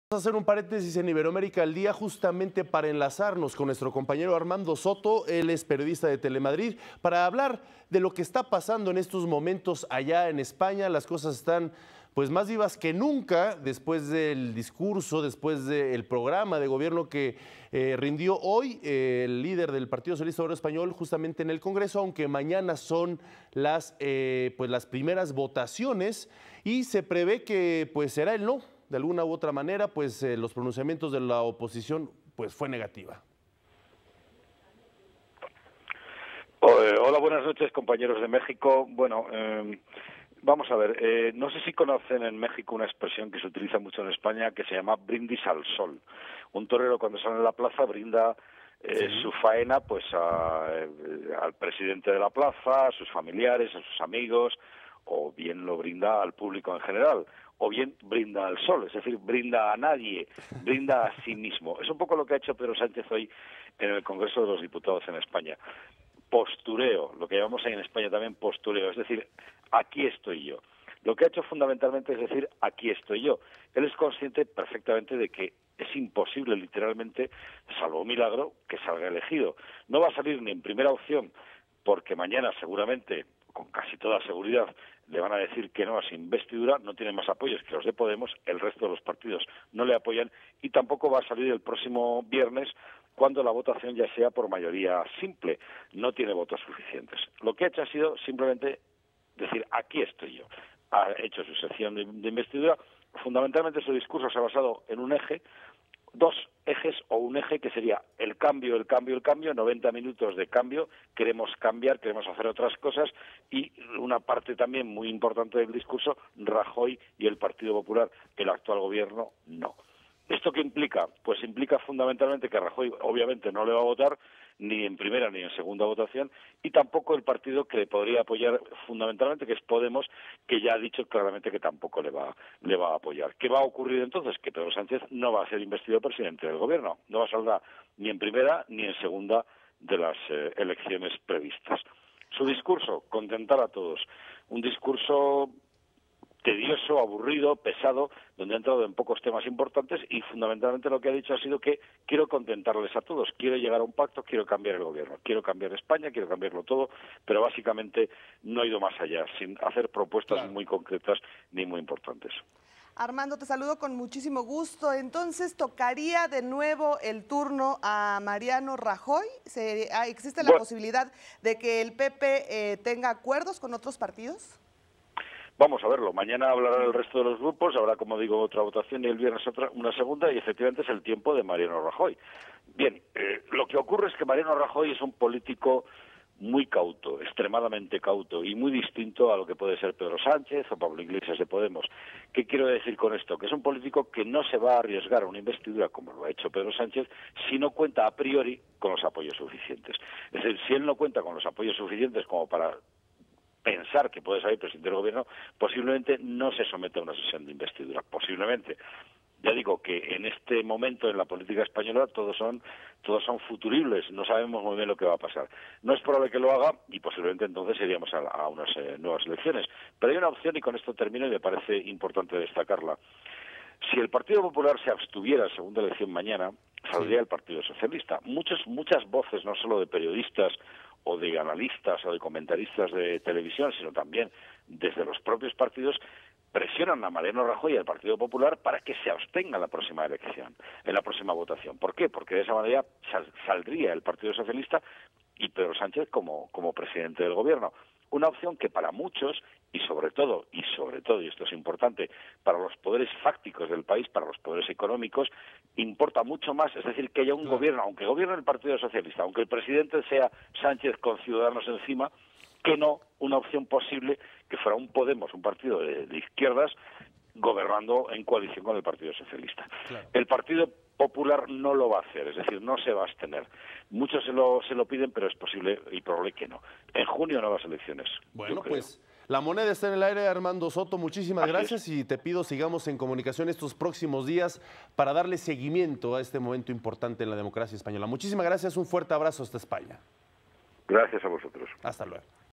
01/03/16 Armando Soto, periodista de Telemadrid
En el Congreso de España inició el debate de investidura de Pedro Sánchez, líder del Partido Socialista Obrero Español, segunda fuerza parlamentaria tras los comicios del 20 de diciembre.
Para hablarnos sobre la situación política que se vive en España, nos enlazamos con Armando Soto, periodista de Telemadrid.